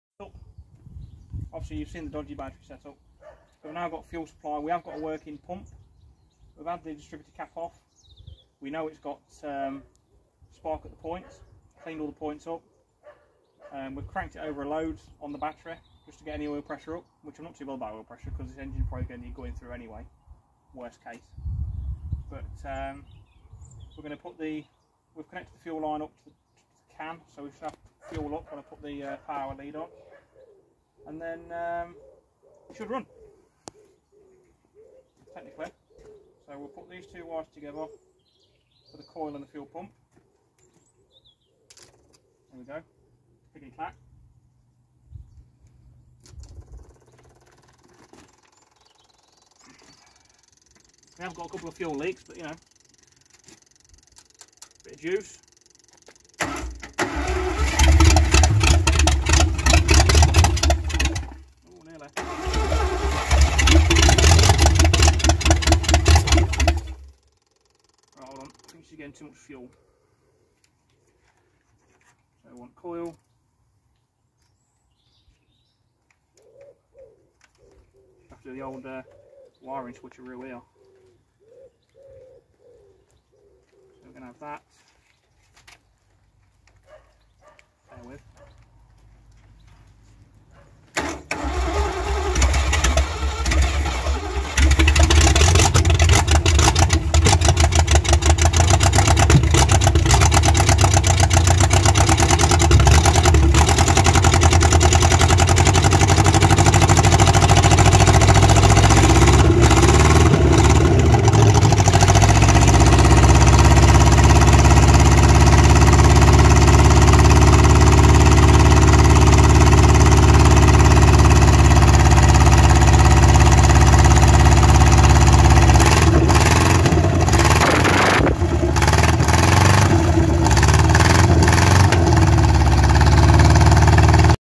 rover p6 3500S from p6 grave yard first start in 15 years part 2
cold start video